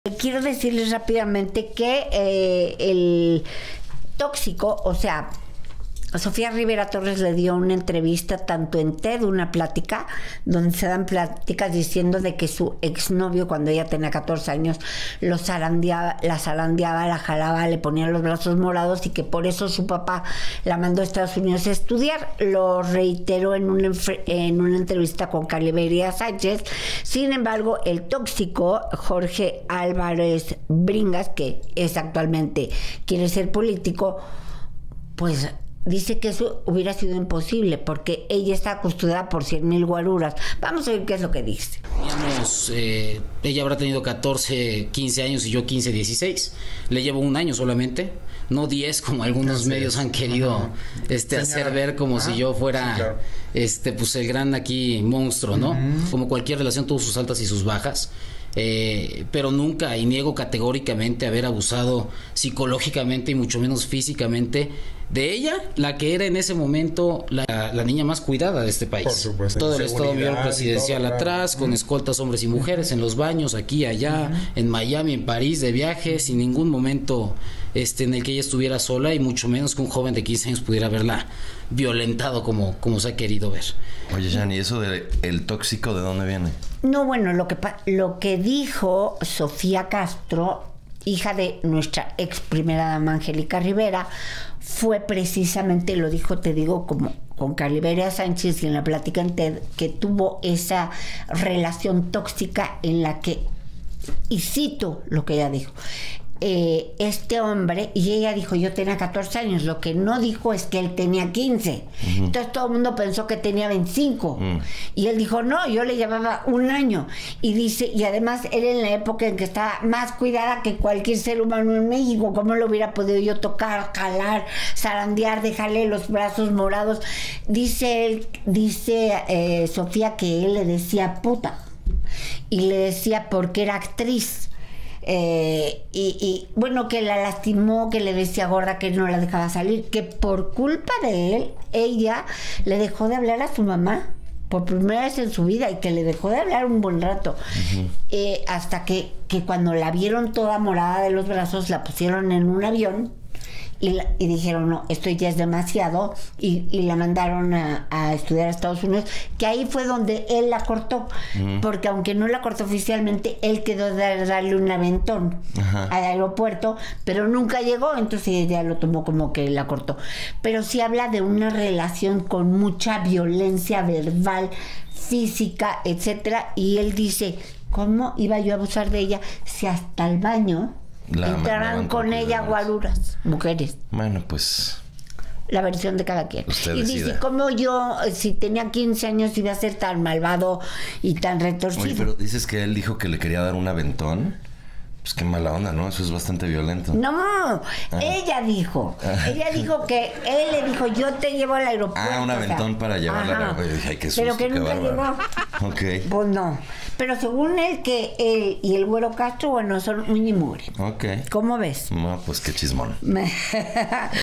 Exnovio de Sofía Castro niega acusaciones de violencia
Las recientes declaraciones de Sofía Castro acerca de la violencia que sufrió por parte de uno de sus exnovios han generado un intenso debate en redes sociales y medios de comunicación. Mientras la actriz no revela el nombre, Jorge Álvarez Bringas ha negado categóricamente ser la persona involucrada. Sigue leyendo para conocer más sobre esta controversia.

Grupo Fórmula

Canal sugerido

Descarga nuestra